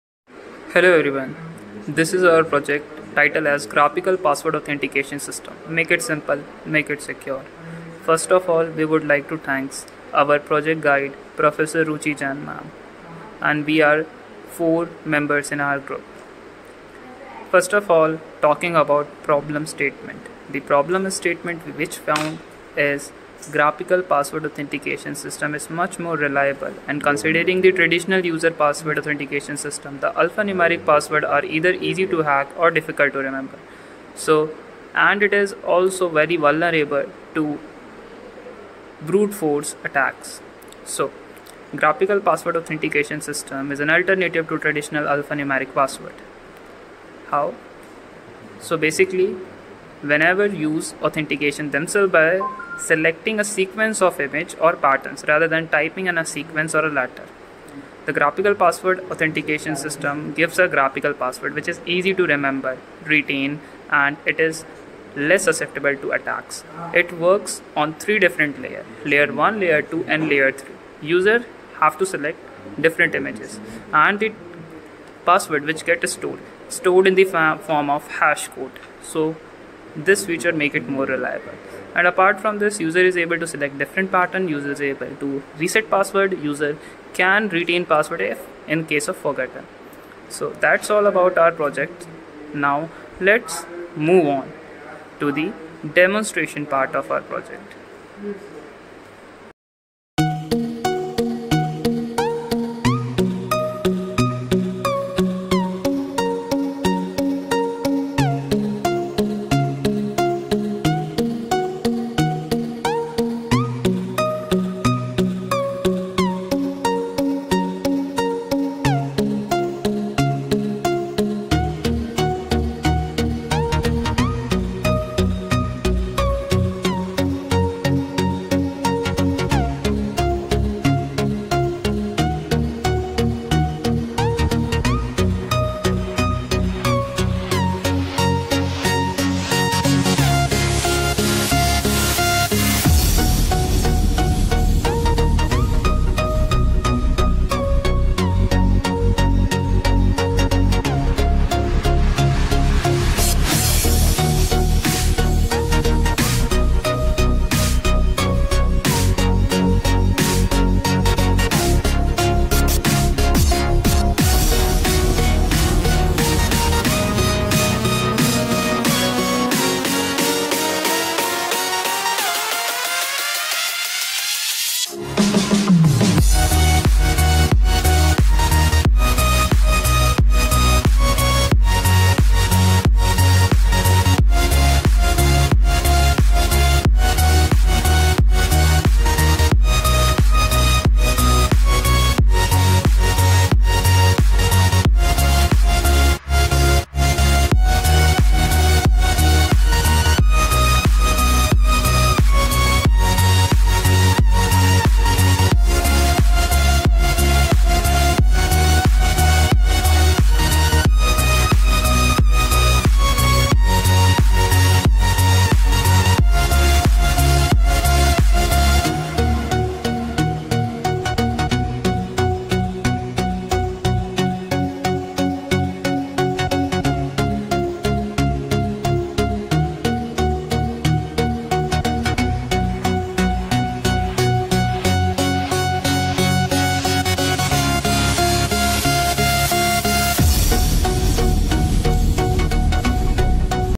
Design & Development of Graphical Password Authentication System | Minor Project | Jan June 2023
Group Members
0187CS201147 Sanjeev Kumar Singh
0187CS201141 Sachin Kumar
0187CS201143 Shahil Chowdahary
0187CS201183 Vivek Verma